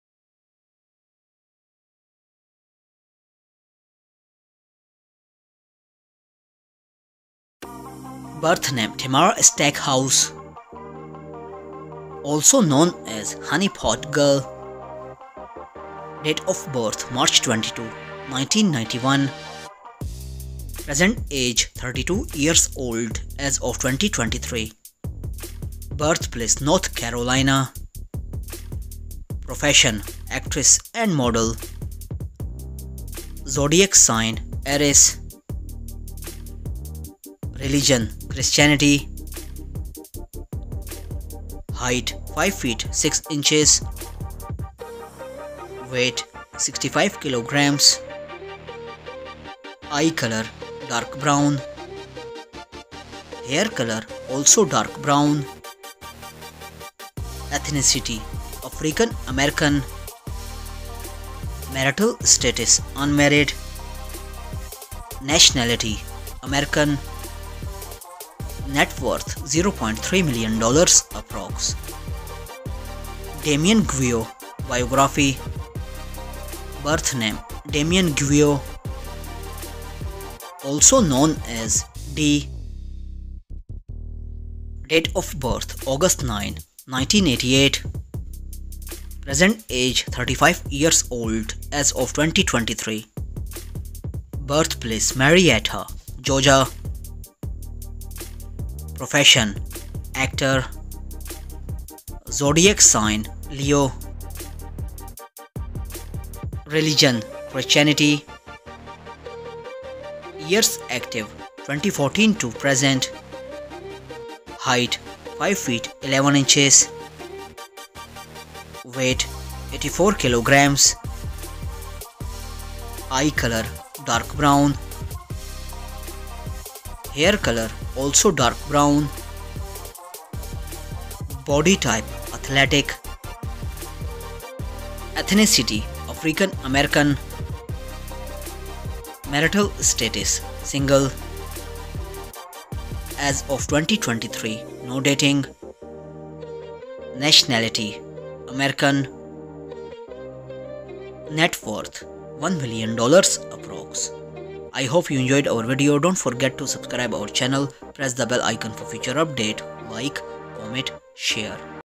Kountry Wayne D And Honey Pot Girl Relationship, Family, Net Worth, Hobbies, Age, Comparison, Facts
Join us as we explore the relationship between Kountry Wayne D and Honey Pot Girl! In this exclusive video, we delve into their family life, net worth, hobbies, age and more. Discover the facts about their relationship and how they compare to each other. From their early beginnings to their rise to fame, we’ve got all the juicy details. Don’t miss out on this chance to get up close and personal with two of the hottest stars in the game.